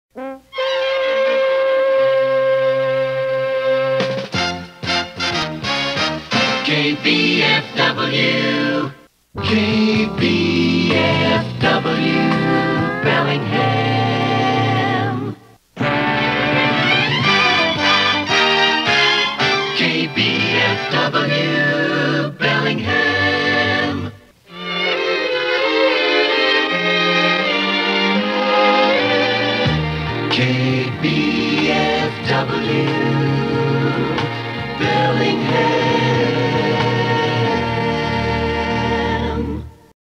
KBFW Jingles (PAMS Productions, Dallas)
The jingles began running on KBFW A.M., country radio 930, in the early 1970's. The accompanying promotional billboards were posted around Bellingham in the late 1970s.